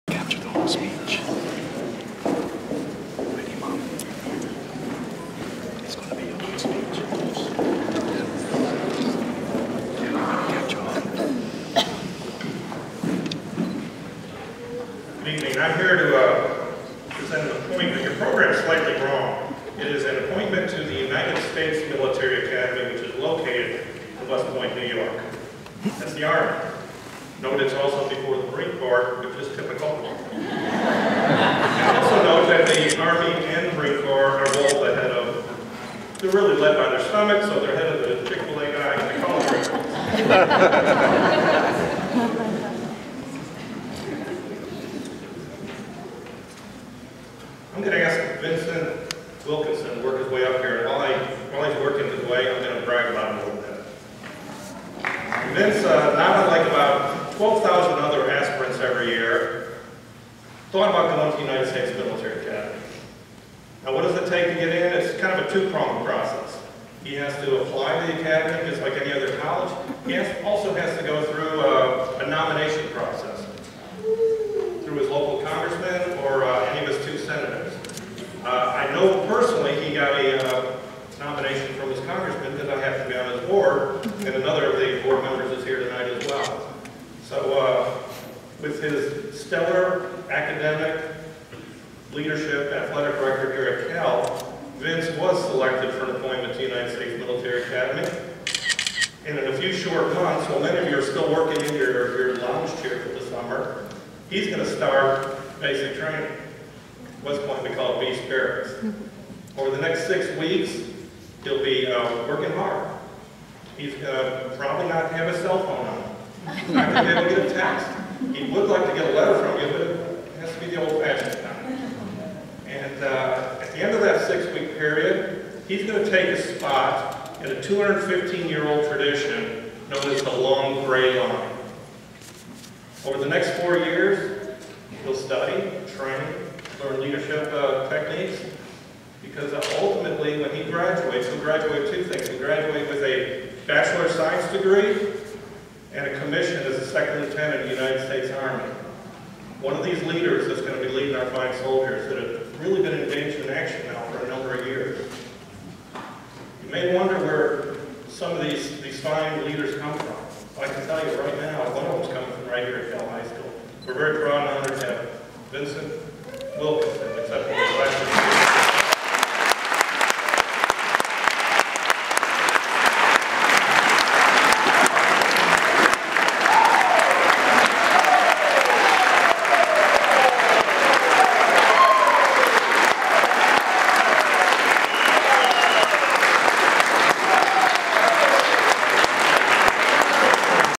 West Point presentation - Vince Wilkinson Kell High School 2017
Major Dale Hajost USMA '82 Officiated  Vince's appointment to West Point during Honors night at Kell High School.